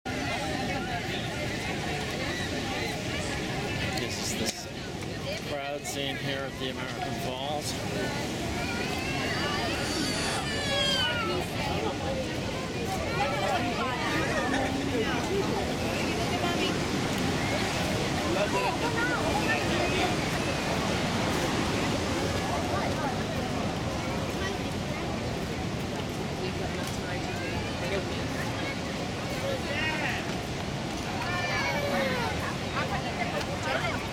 crowd scene at Prospect Point near the American Falls, Niagara Falls State Park, Niagara Falls, NY
This is part of the large crowd gathered relatively late in the evening in Niagara Falls State Park near the American Falls of Niagara Falls in Niagara Falls State Park on July 5, 2018.